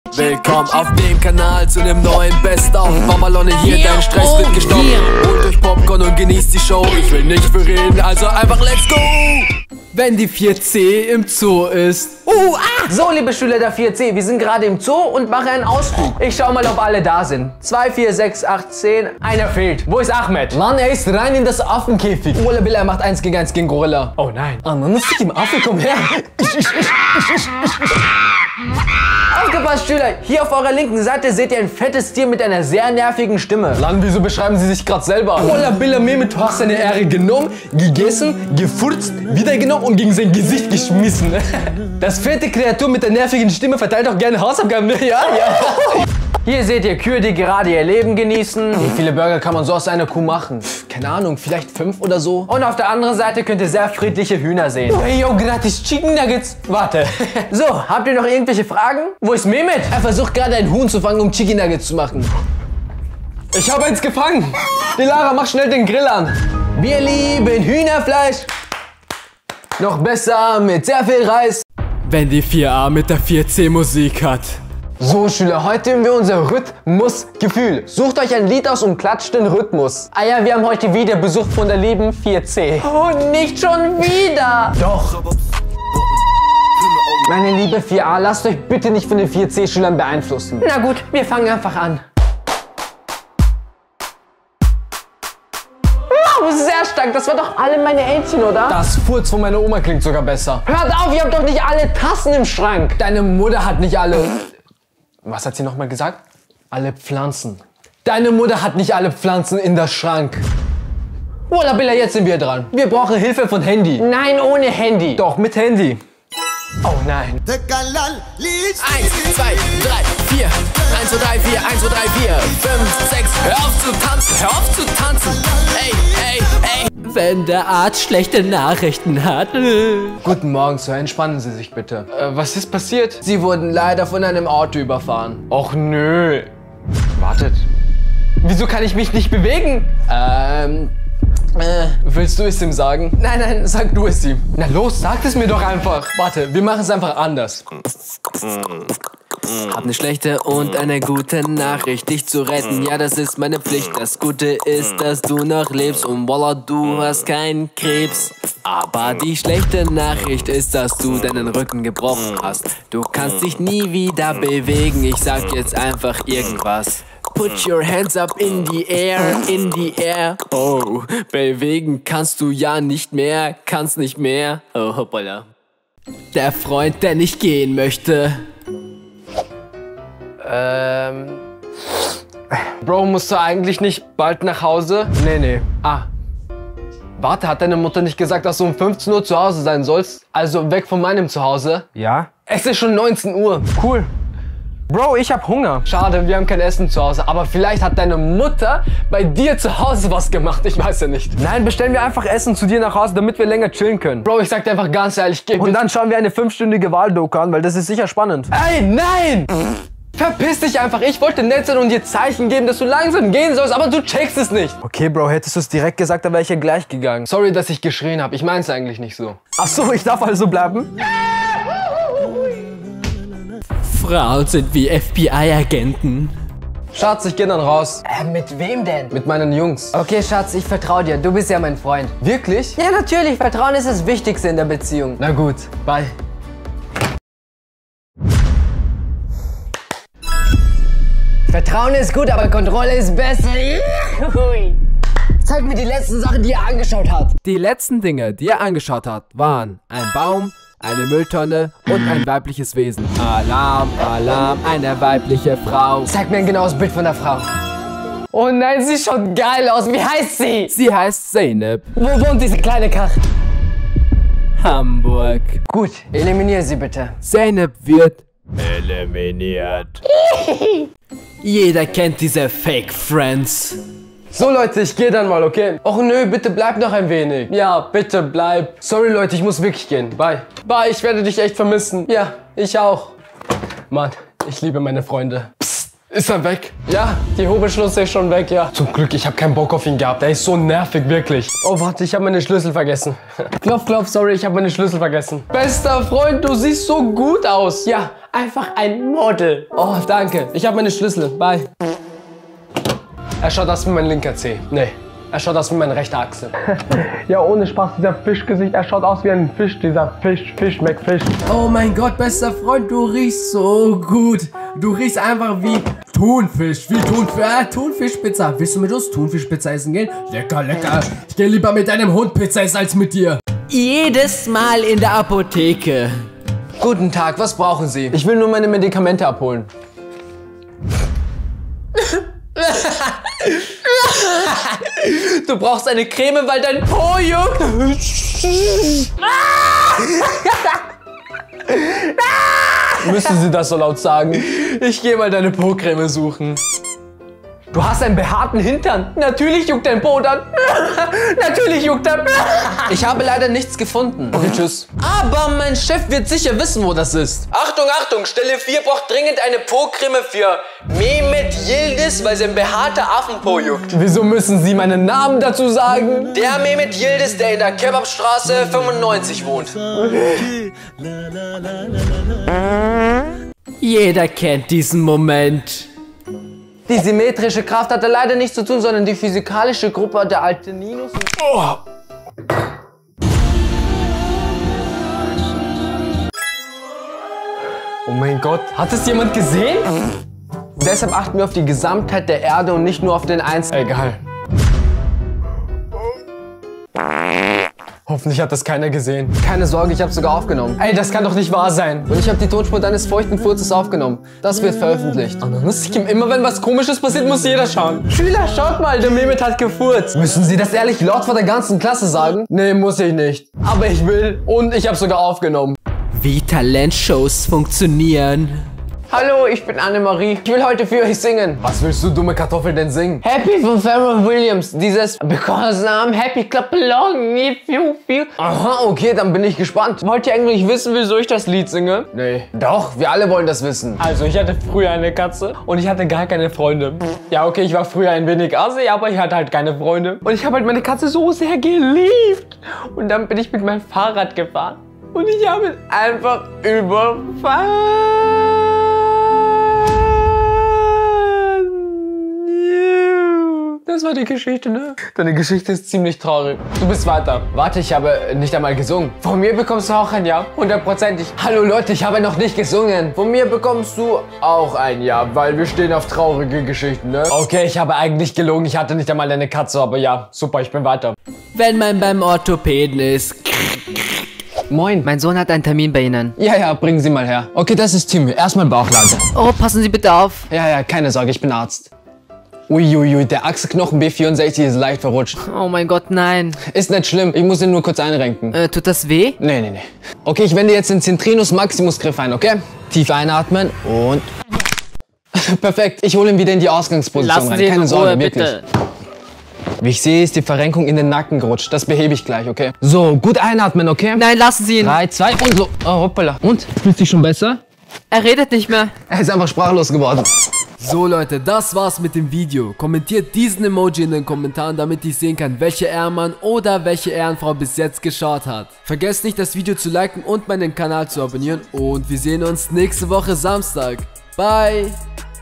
4A und 4C im ZOO | Welche Klasse ist besser? 😱😂 (BEST OF) | Mohi__07
🍫 Yallah Bye